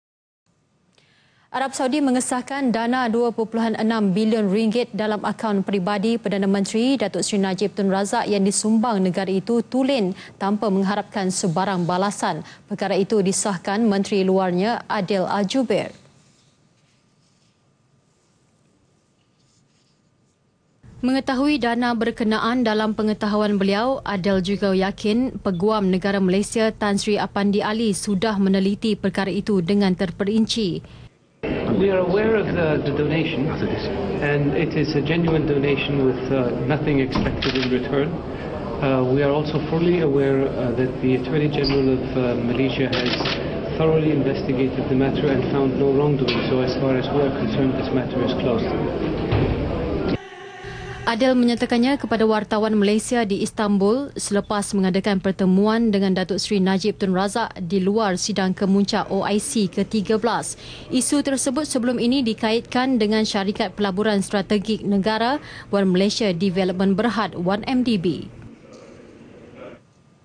DANA RM2.6 BILION - ARAB SAUDI SAHKAN SUMBANGANNYA [15 APRIL 2016]
Arab Saudi mengesahkan, dana 2.6 bilion ringgit dalam akaun peribadi Perdana Menteri, Datuk Seri Najib Tun Razak yang disumbang negara itu tulen, tanpa mengharapkan sebarang balasan.

Perkara itu disahkan Menteri Luarnya, Adel Al-Jubeir.